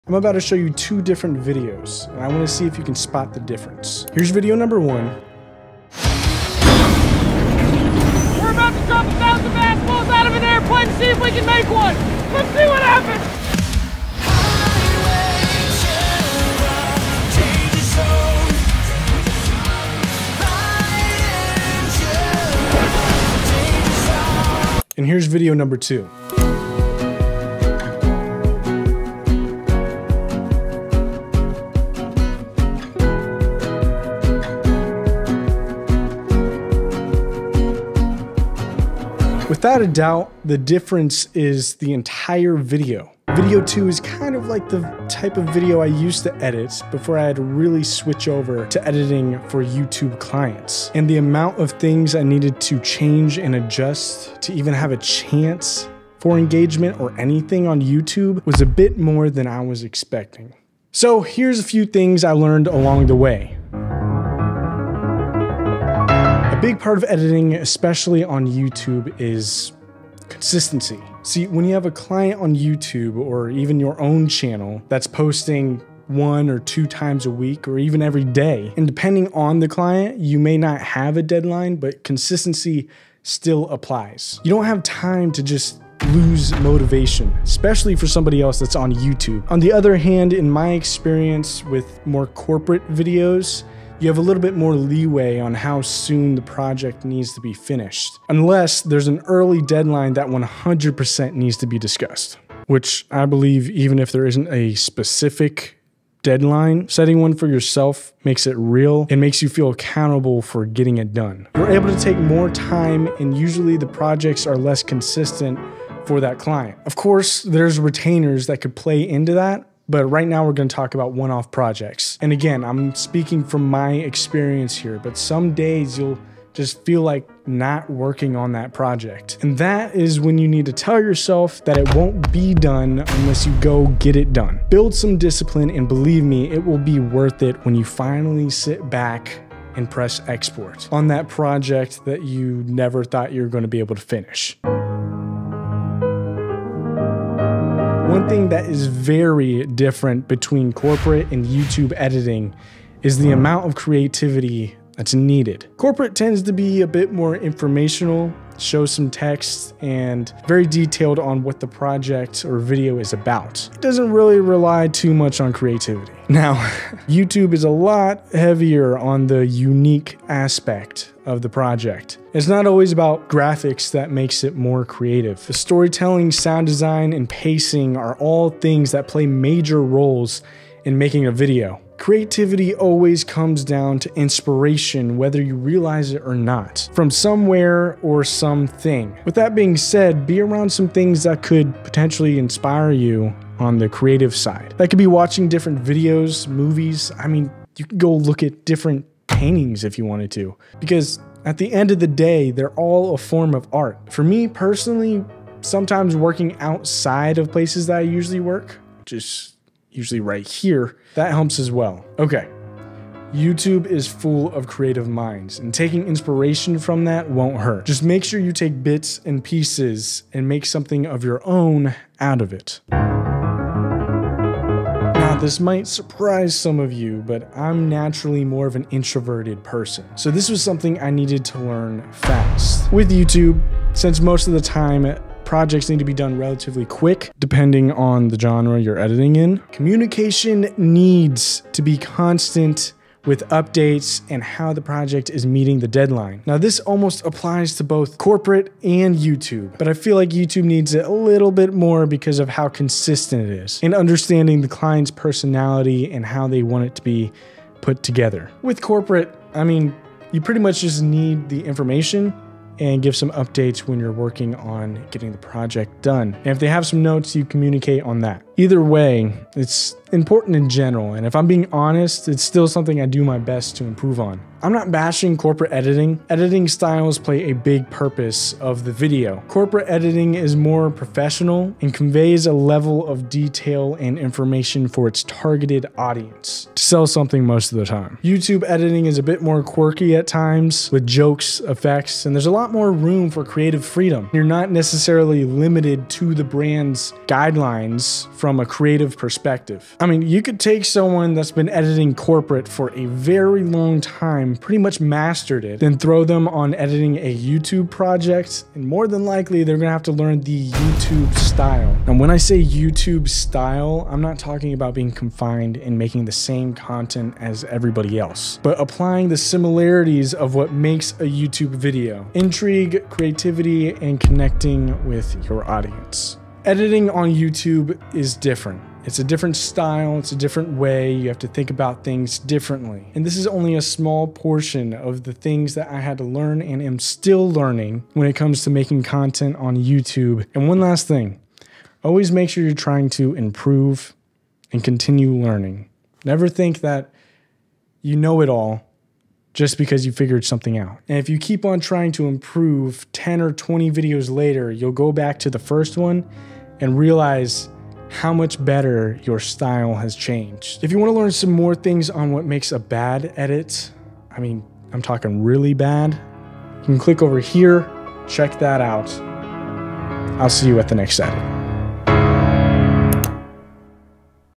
Why Editing for YouTubers is Different
I used to edit corporate, then I switched to editing on youtube and the change was drastic. So I decided to make a video about the process from editing company videos to editing for YouTubers...